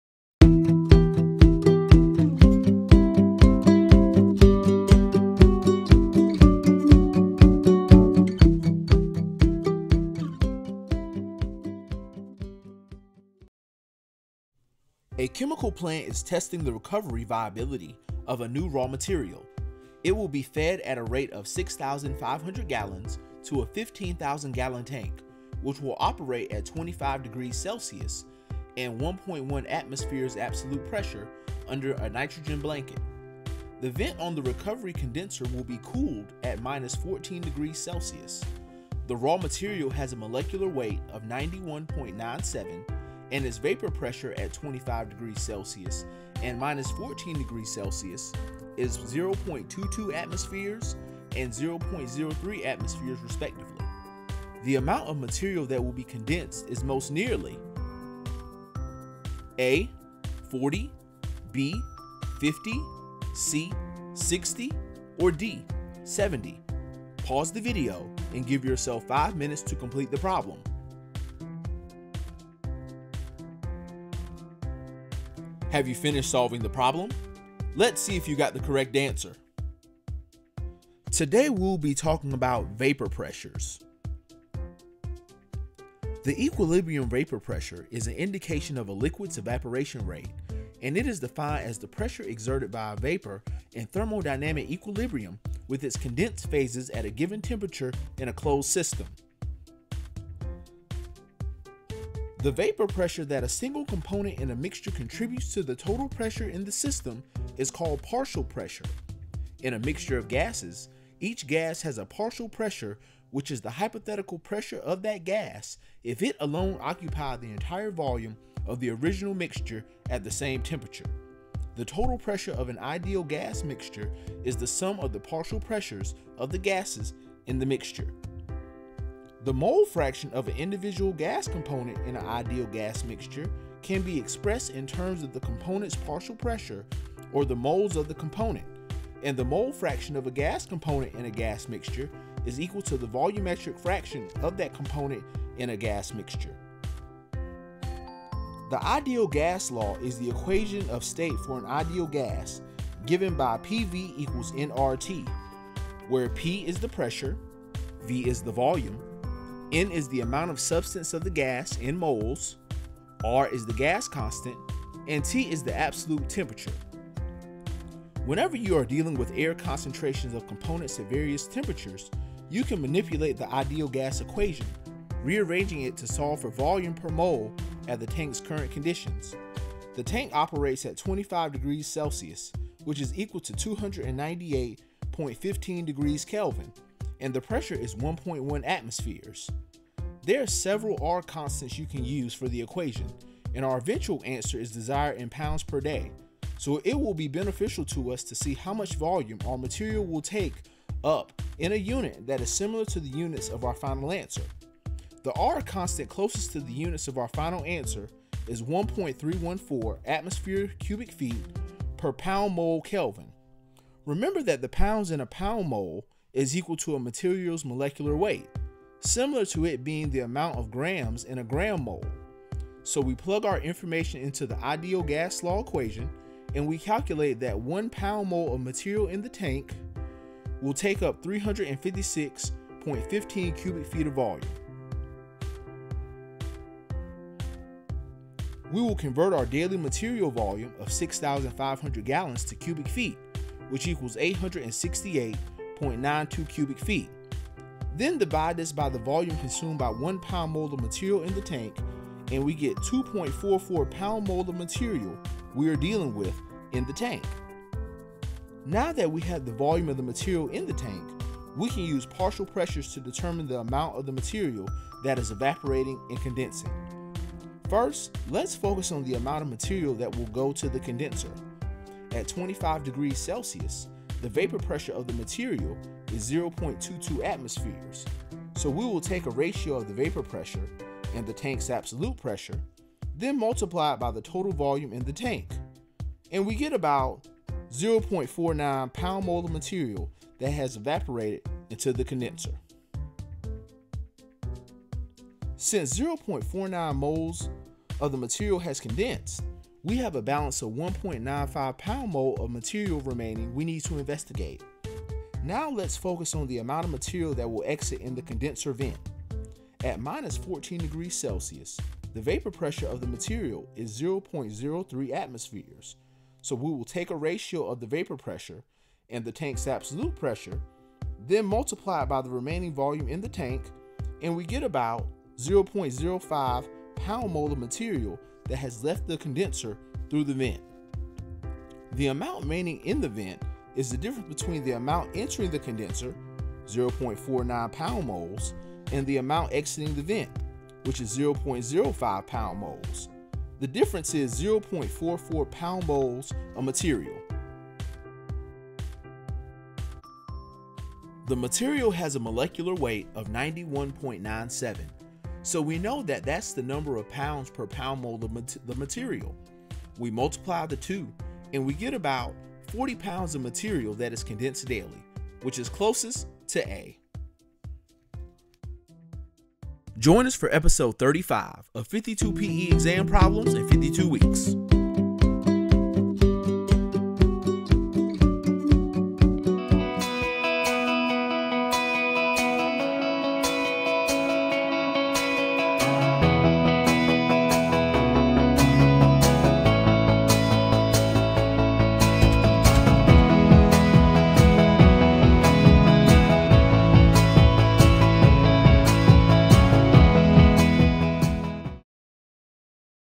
Vapor Pressures - Episode 34 (Air Pollution Engineering) | 52 PE Exam Problems in 52 Weeks
Note: From 3:59 to 4:19, the first line in yellow should show 356.15 cubic feet/lb mol instead of 356.15 L/lb mol.

Thanks to everyone for your views, as well as your comments & suggestions to make the series better for us all. We also do group & individual tutoring sessions for the PE Exam, so if you are interested in our offerings let us know and we can see how we can be of service to you!